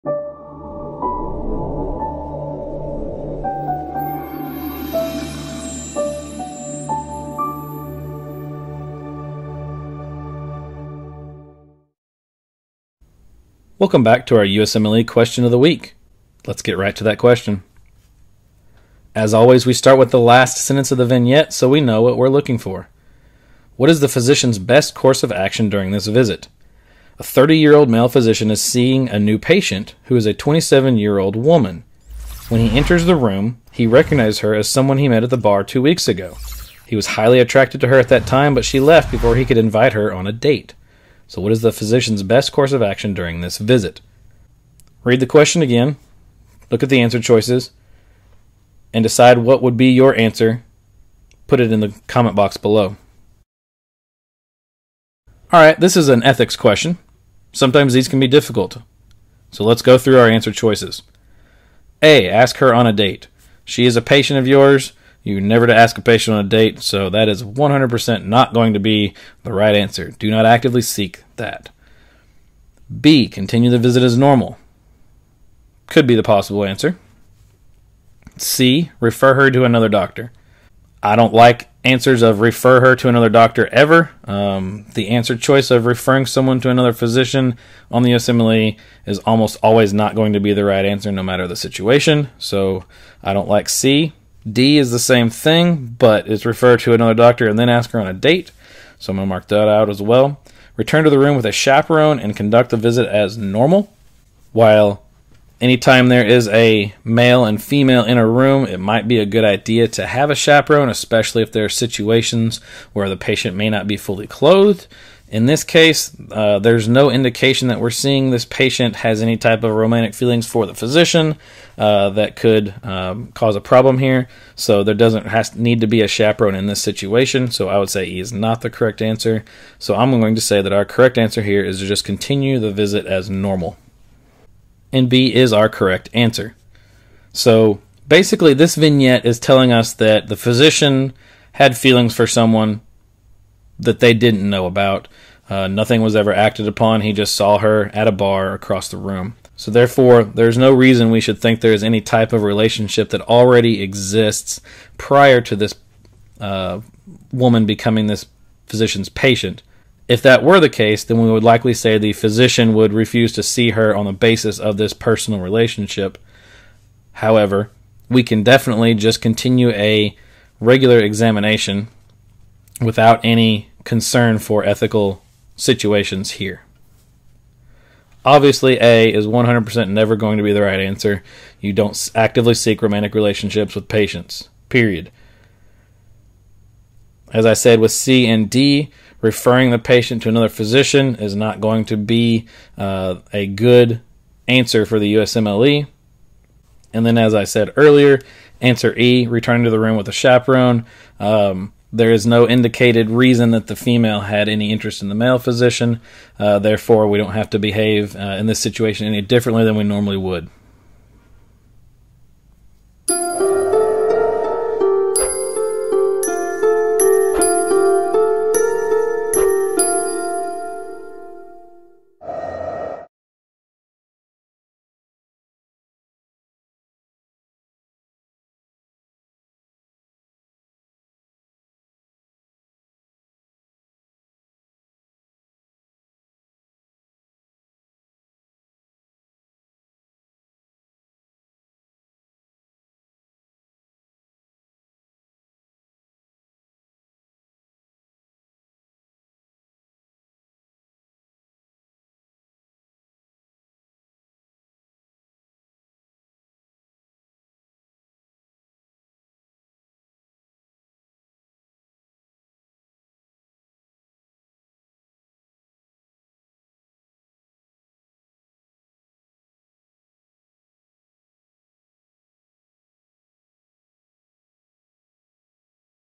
How to Break Down a Question | USMLE Step 1 | Ethics
How to break down a USMLE Step 1 question, Cardiovascular from MedSmarter with Dr. Jason Colip.

The attached video is the breakdown of a question regarding Ethics.

A 30-year-old male physician is seeing a new patient, who is a 27-year-old woman. When he enters the room, he recognizes her as someone he met at a bar 2 weeks ago. He was highly attracted to her at that time, but she left before he could invite her on a date. What is the physician’s best course of action during this visit?